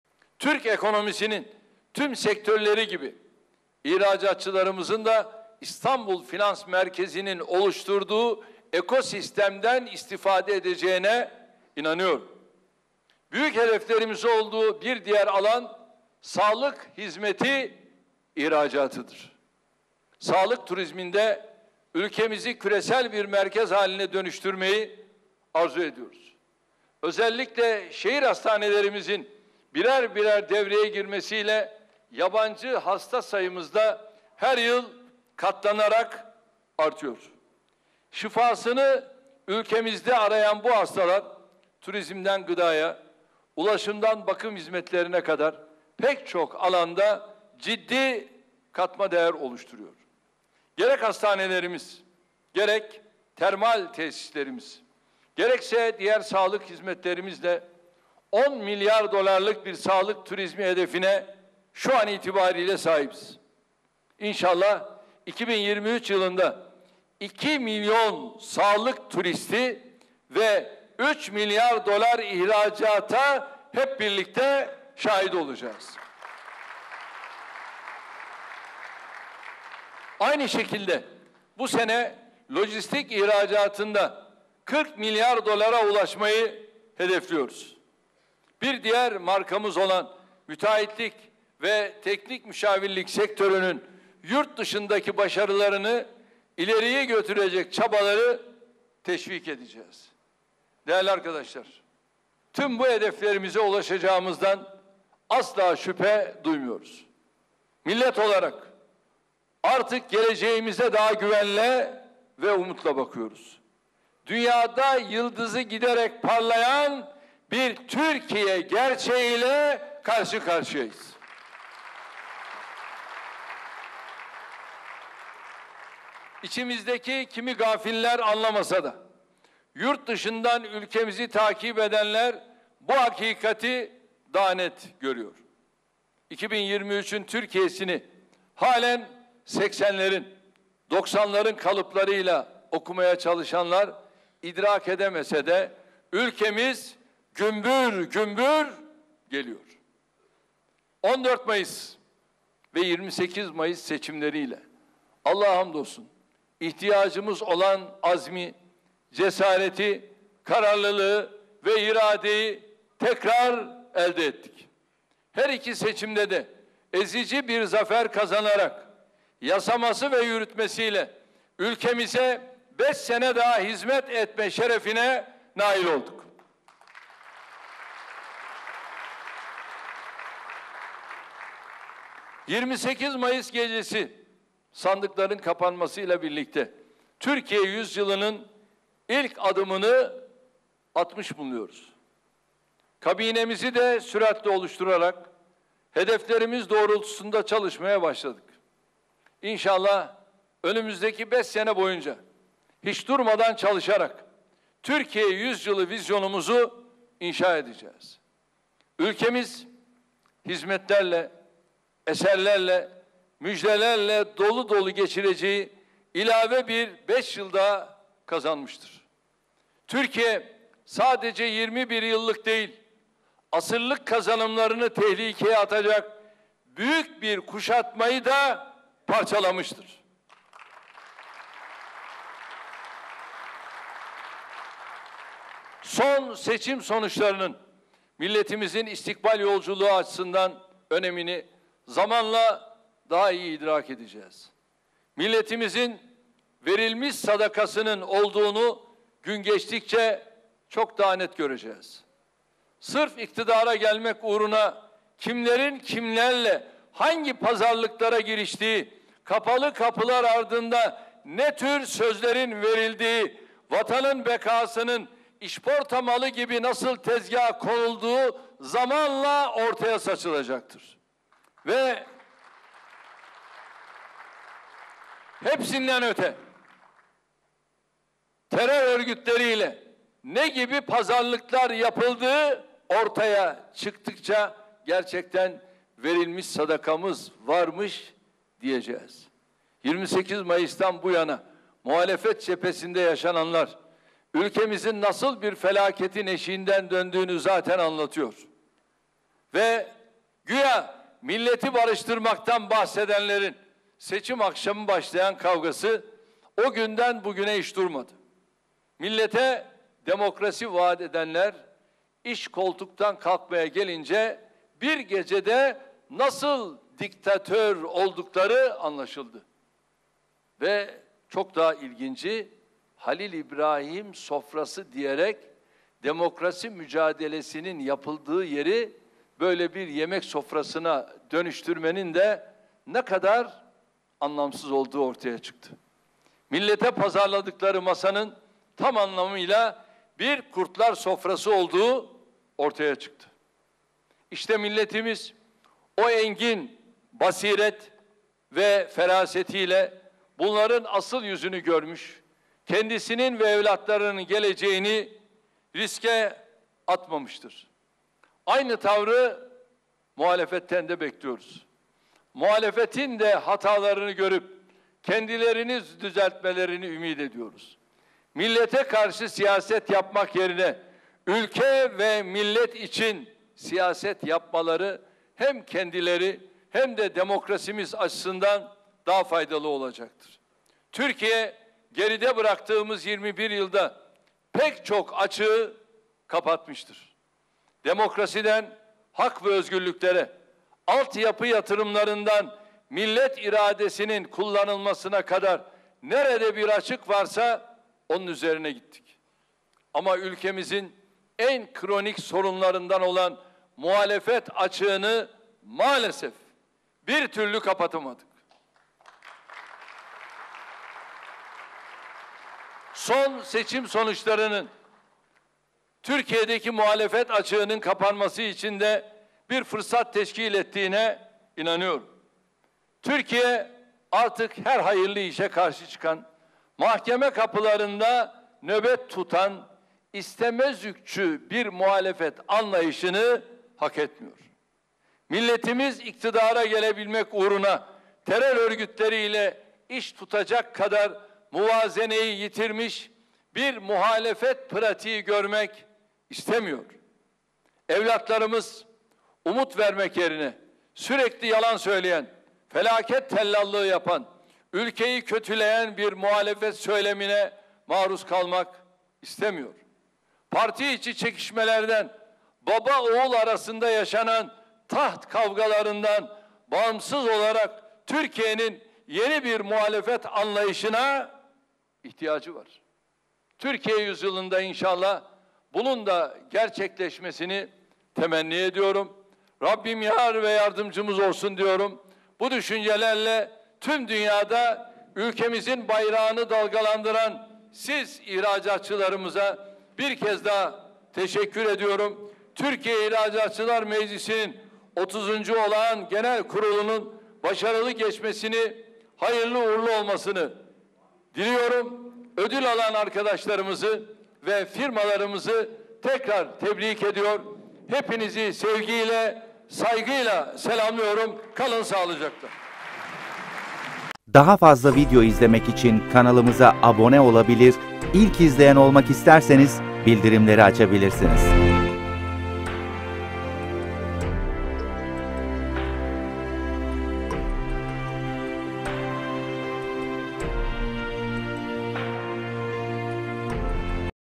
SONDAKİKA Erdoğan'dan CHP'ye Çok Sert Sözler! "Seçimden Beri Kavgaları Bitmedi"
Cumhurbaşkanı Recep Tayyip Erdoğan Yaptığı Genel Kurul konuşmasında muhalefeti sert bir dille eleştirdi. "Milleti barıştırmaktan bahsedenlerin seçim gecesinden beri kavgası hiç bitmedi" şeklinde konuştu.